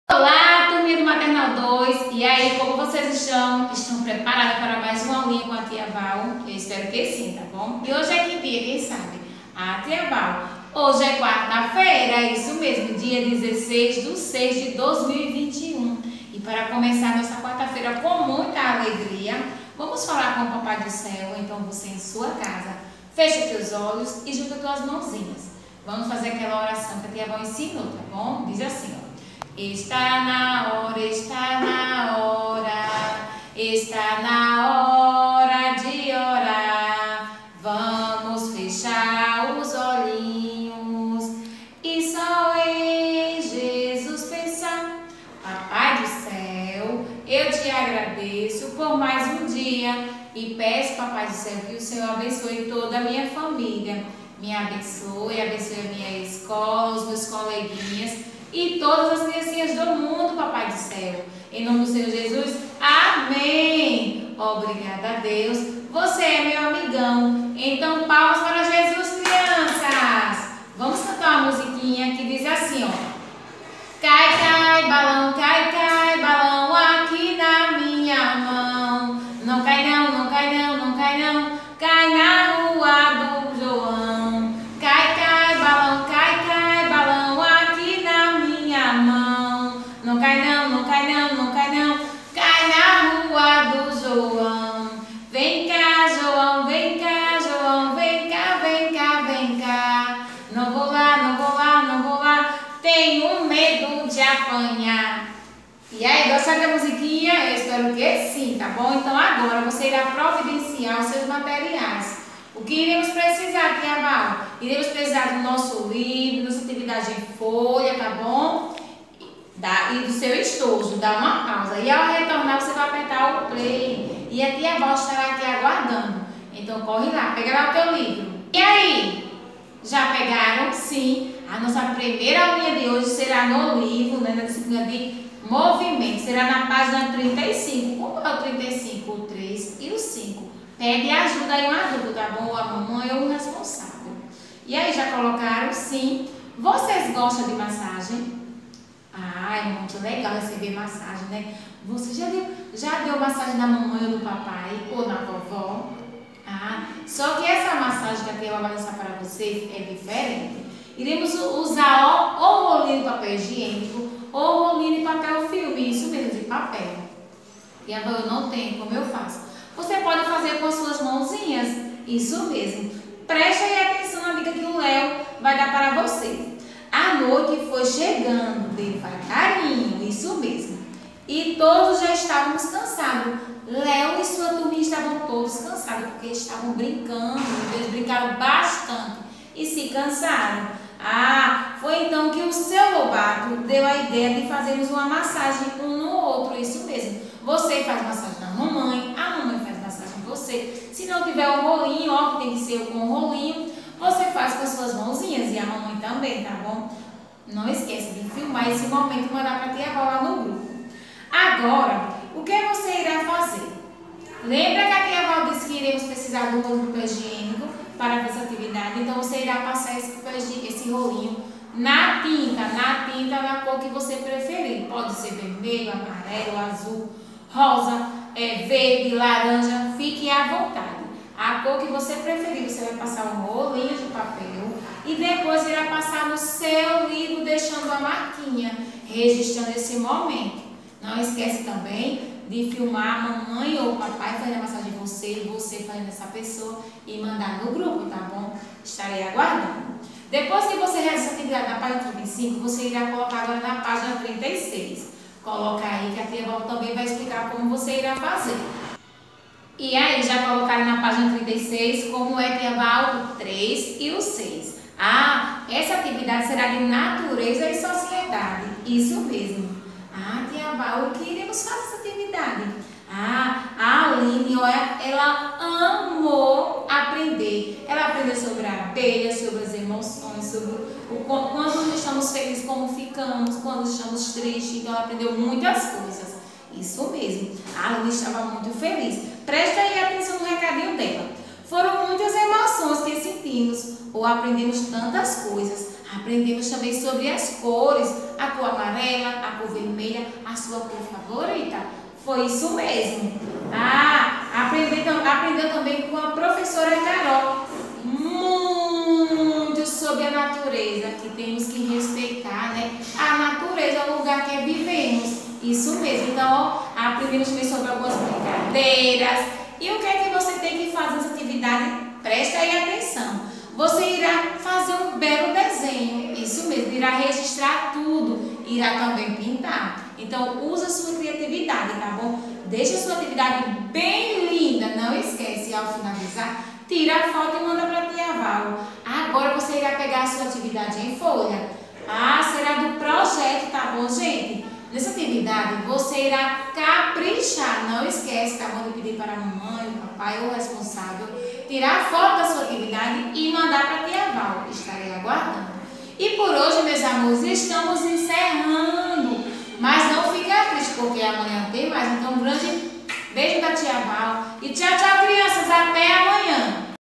Maternal 2: Aula -16/06/2021
Aula da turma do Maternal 2 com a tia Val.♥️